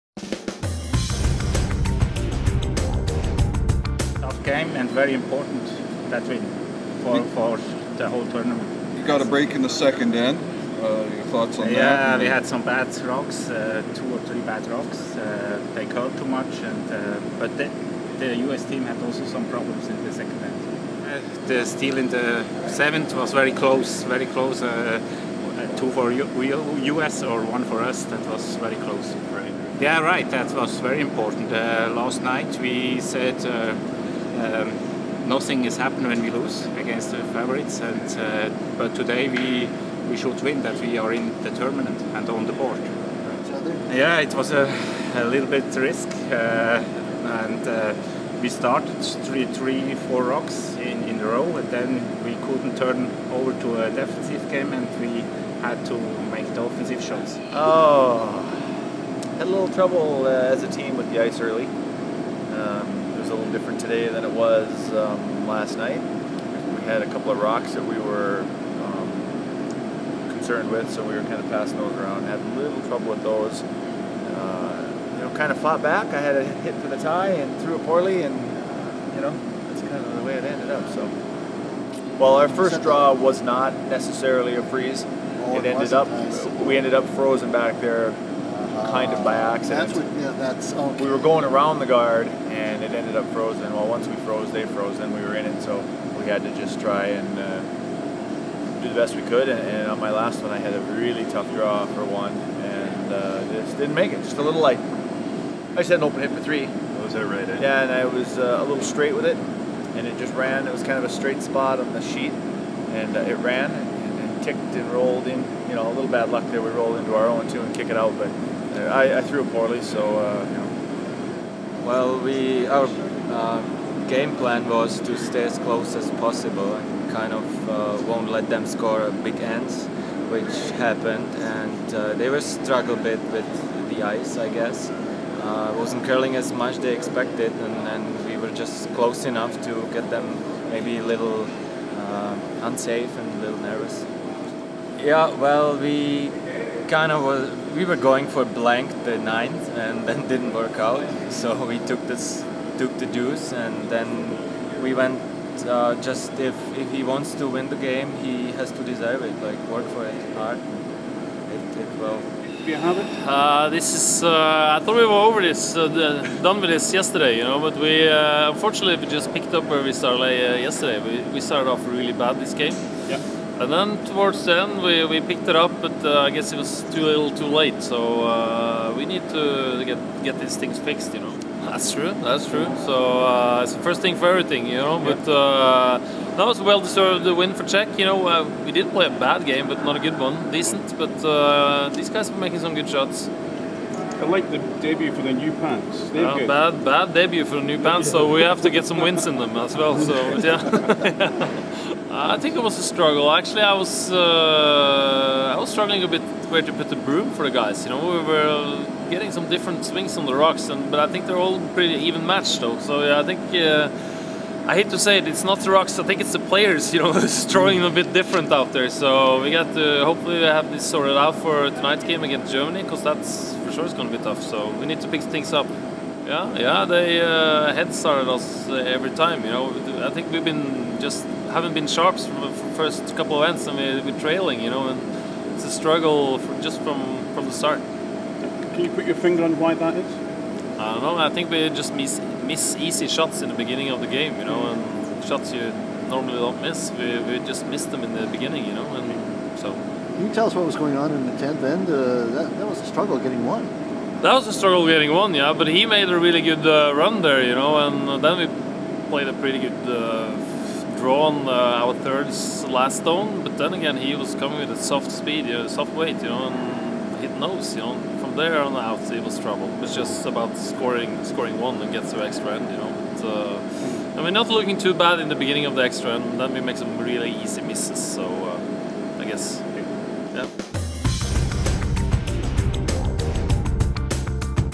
2011 Ford World Men's Curling Championship - Draw 3 Media Scrum
Post game interview with Jiri Snitil, Christof Schwaller, Pete Fenson and Thomas Ulsrud.